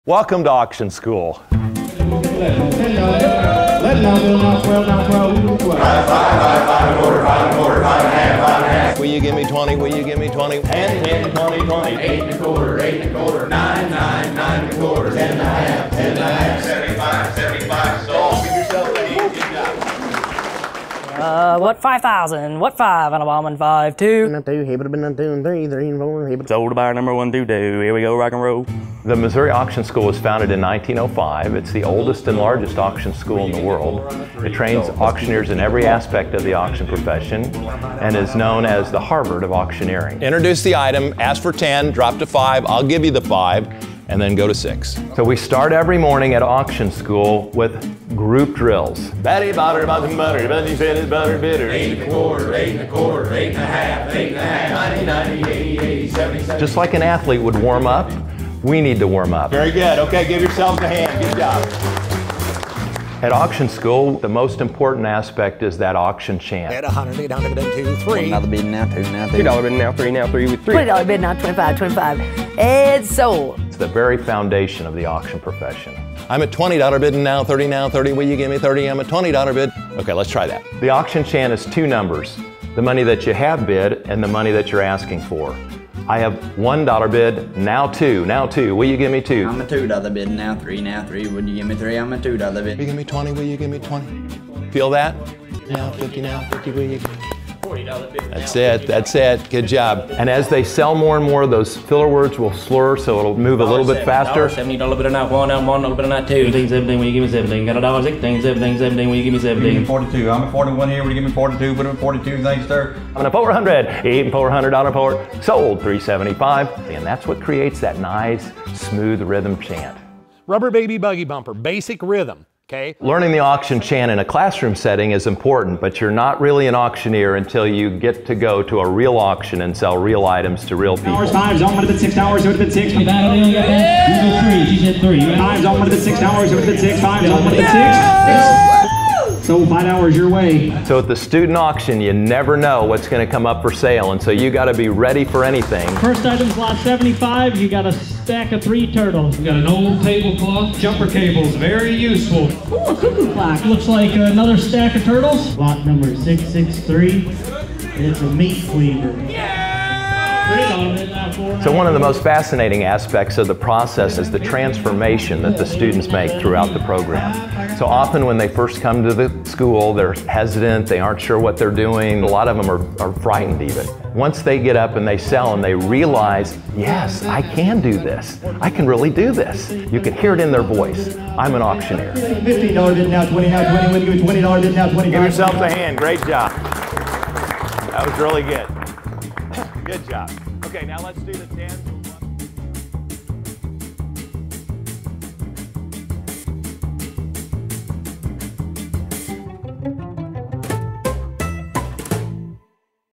Perfect Your Speed Talking at This Auction School
The Missouri Auction School is the oldest and largest auction school in the world. There, students learn all aspects of the business, but most importantly, they hone their auction chant. Before students graduate they have to sell at a real auction for real money. Going once, going twice ... sold!